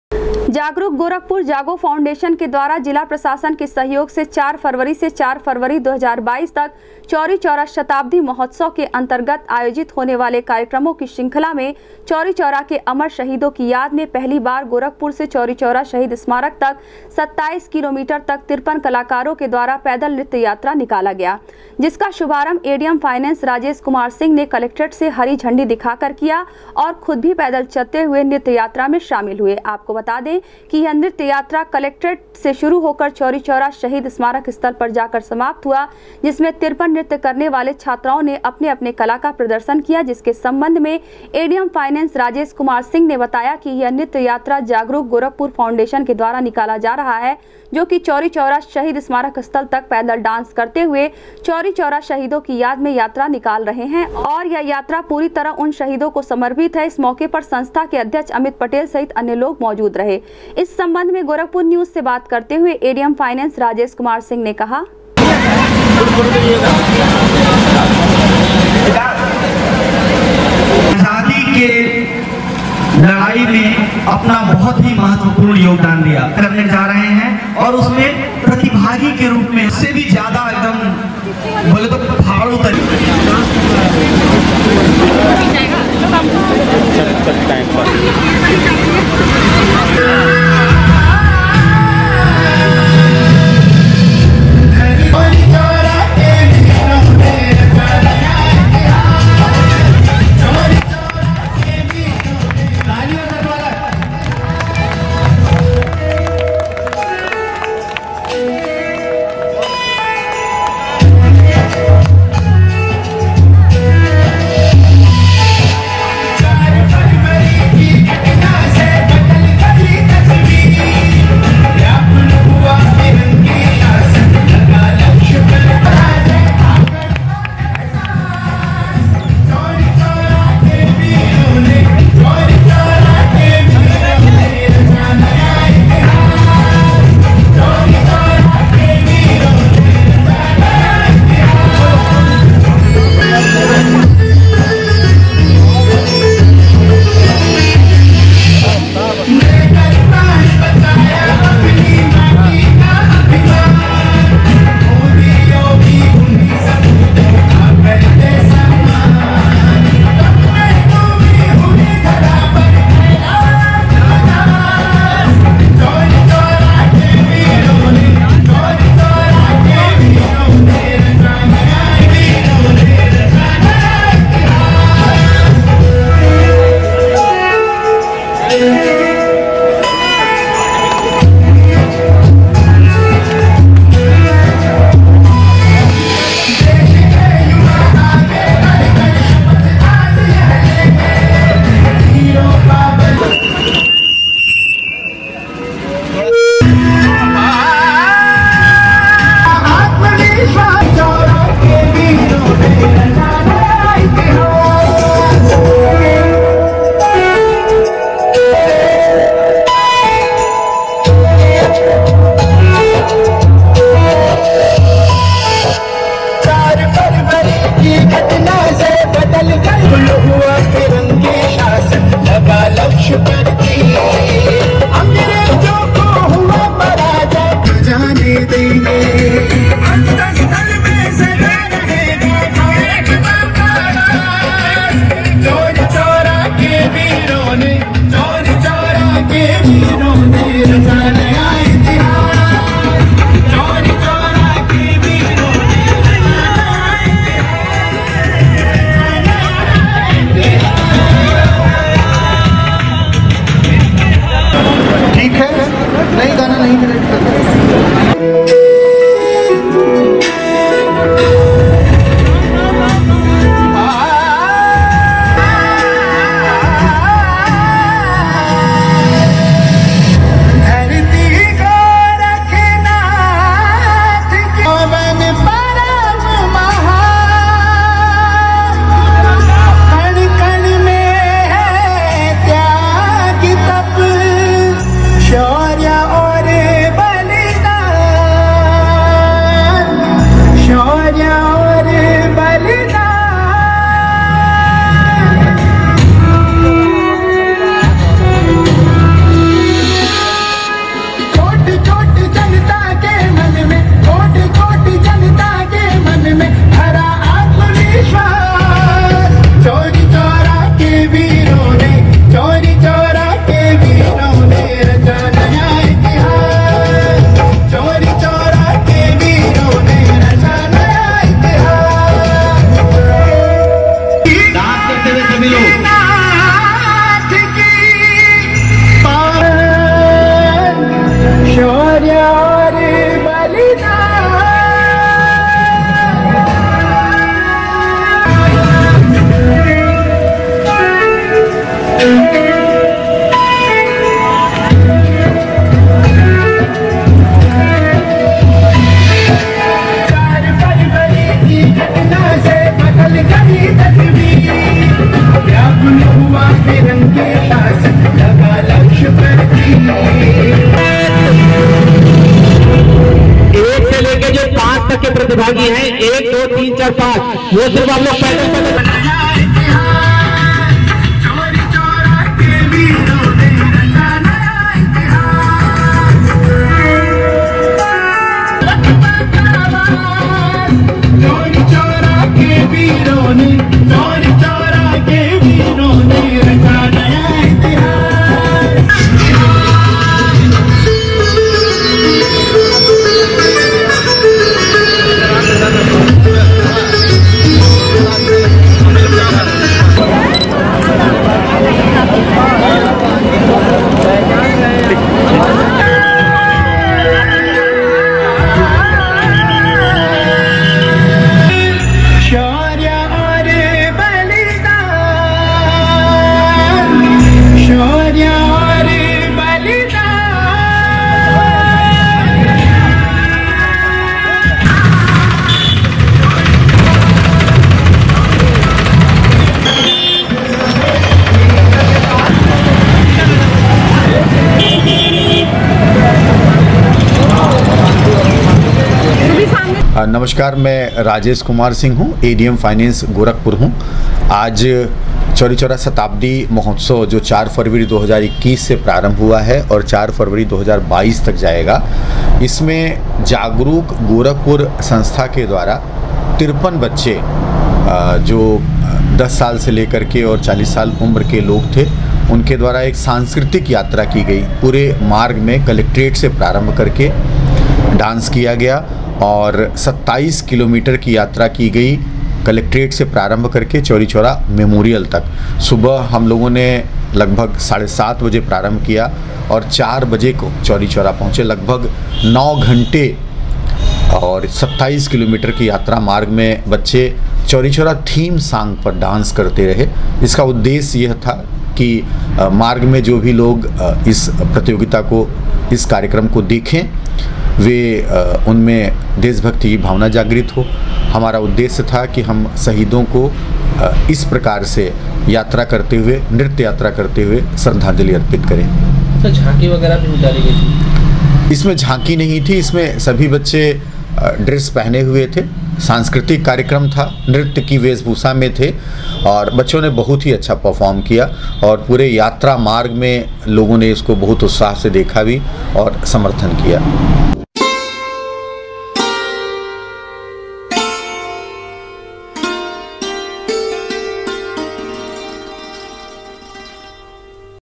जागरूक गोरखपुर जागो फाउंडेशन के तत्वावधान में चौरीचौरा शहीदों की याद में डीएम कार्यालय से चौरीचौरा
जागरूक गोरखपुर जागो फाउंडेशन के तत्वावधान में चौरीचौरा शहीदों की याद में डीएम कार्यालय से चौरीचौरा शहीद स्मारक स्थल तक 53 कलाकारों के द्वारा निकाली गयी 27 किलोमीटर तक पैदल नृत्य यात्रा,एडीएम फाइनेंस ने झंडी दिखा कर पैदल नृत्य यात्रा को किया रवाना,खुद भी पैदल पहुॅचे चैरी चैरा,शहीदो को किया नमन